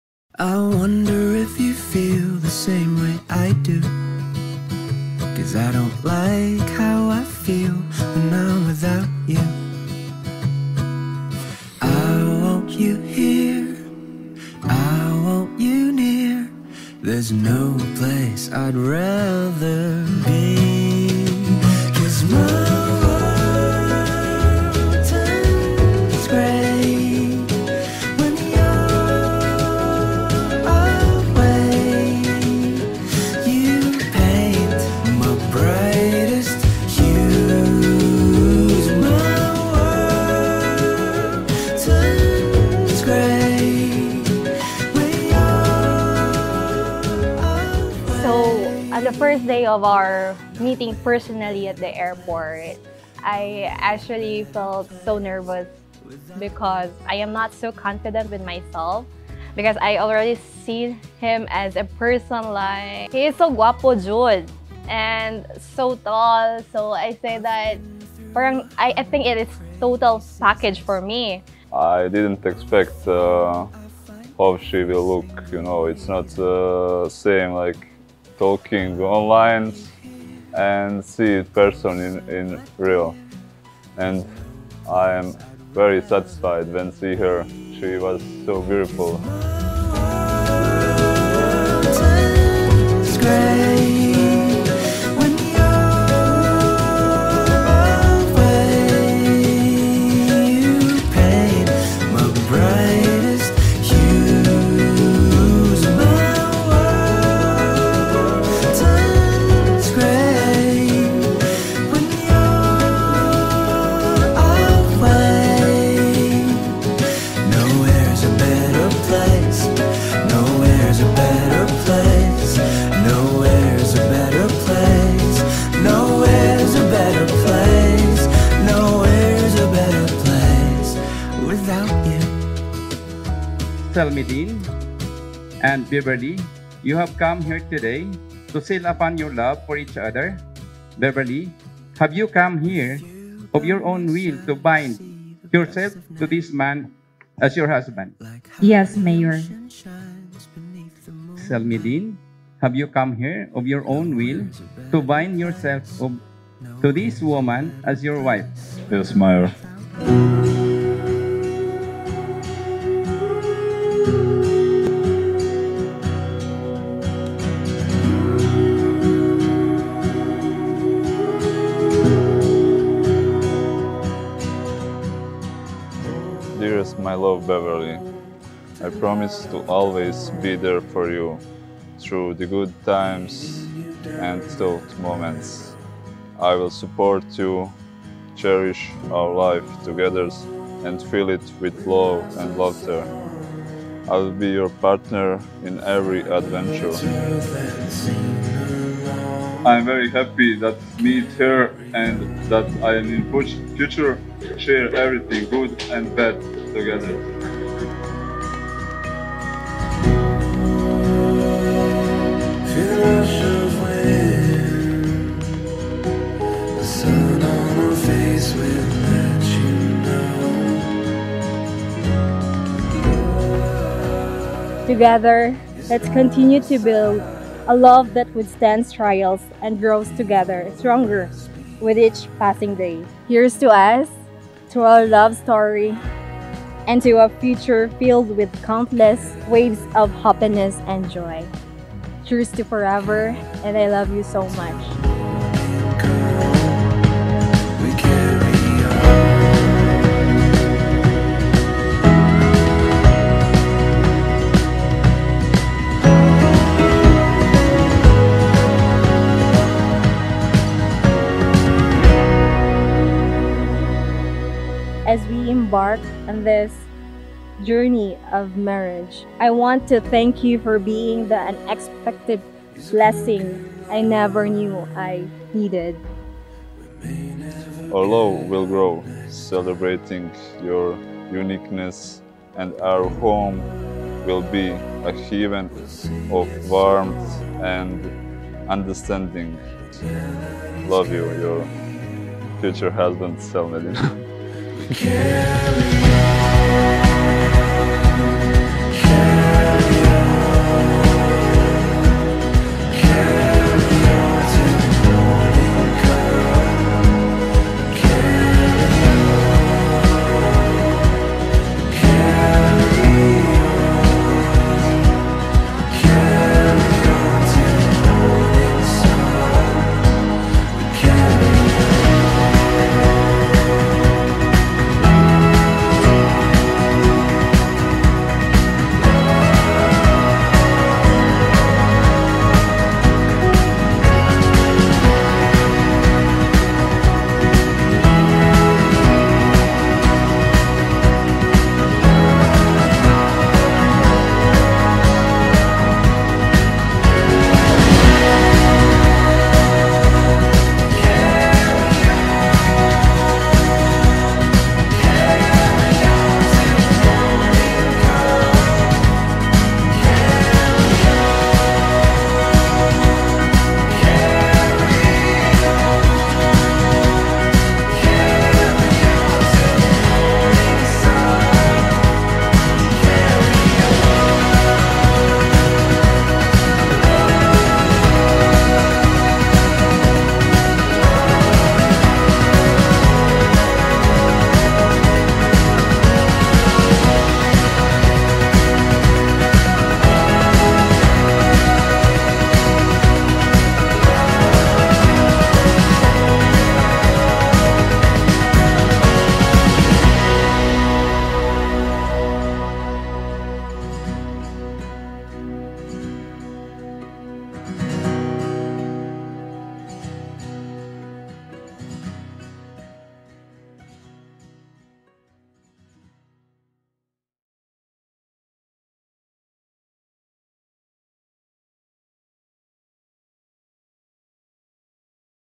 BEVERLY AND SELMEDIN - WEDDING VIDEO HIGHLIGHTS
From the Philippines to Bosnia and Herzegovina, their love story knows no distance. Celebrating the union of Beverly and Selmedin, where East meets West in the name of love. A Filipina beauty and a Bosnian charm, now united as one. Cheers to the newlyweds, Beverly and Selmedin! 🇵🇭❤️🇧🇦

BEVERLY & SELMEDIN - WEDDING VIDEO HIGHLIGHTS

HMUA: @Miss eve
Photo and Video: Shane Aldrich Bonucan Quimno , Nicole Pauline Arocha
Video Director: Prince Dolor's
Wardrobe: Roy Dy Couture
Photobooth: SAP Photobooth
Event Styling: Edsel Granaderos
Venue: MCC HOTEL - Mindanao Civic Center